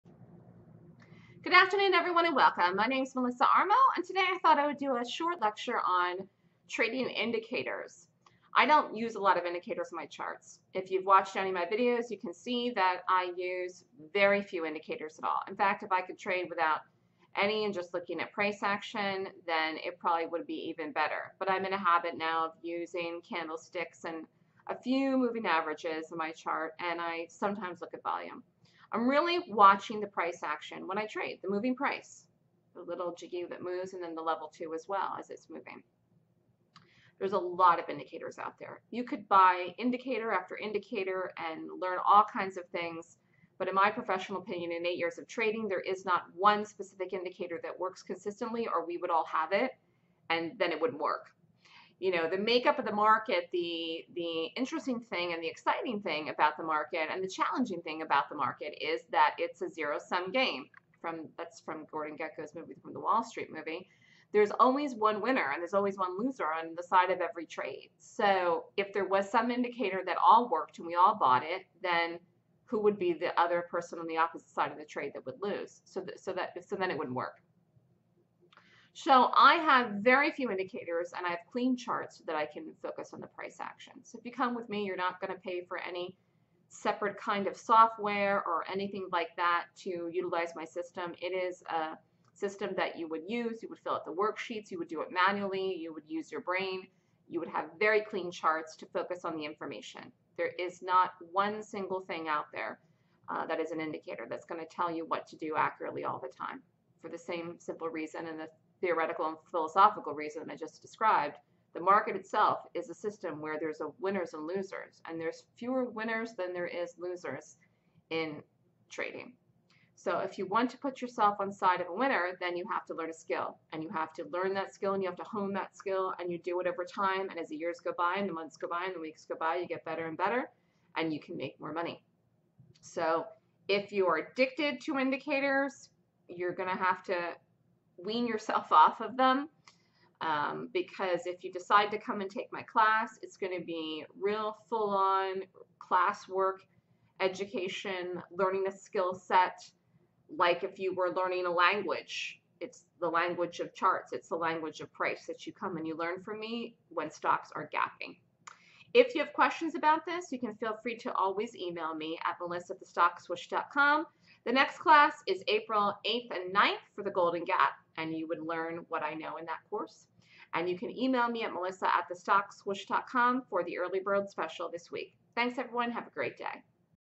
Price Action or Trading Indicators? Focus On The Gap
- Melissa Armo answers the age old question - price action or trading indicators?